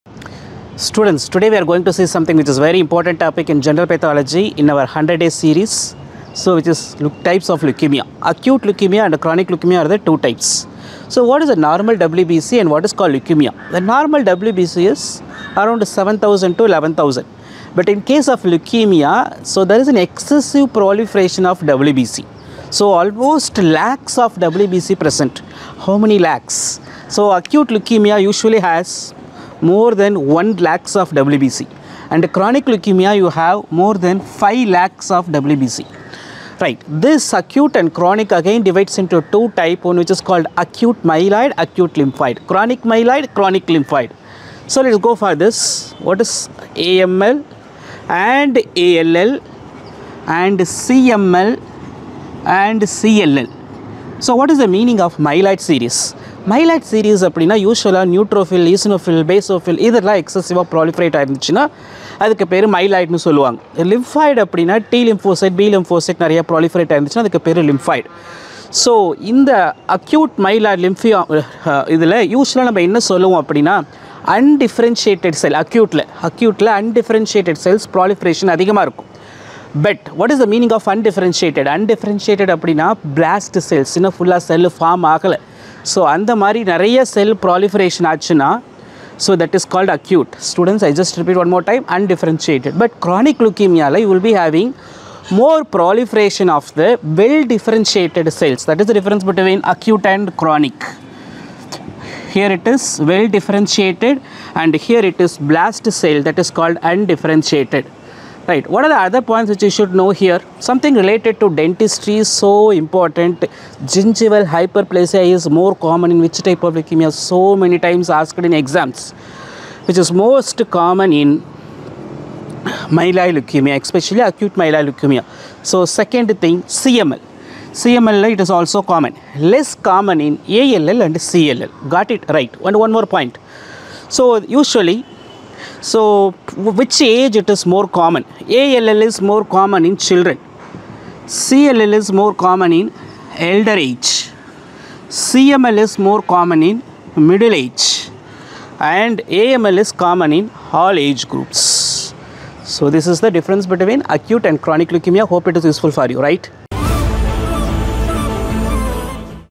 Leukemia in one minute | Day 25 | 100 Days AIR Series | NEET MDS 2026 | ArdentMDS
🩸 Leukemia in One Minute! | Day 25 of the 100 Days AIR Series 🚀

Quick, high-yield concepts explained in just 60 seconds for NEET MDS 2026 aspirants.

📚 Stay consistent with ArdentMDS and cover the most important exam topics every day!

✅ Perfect for rapid revision
✅ Concept clarity in less time
✅ Smart prep for NEET MDS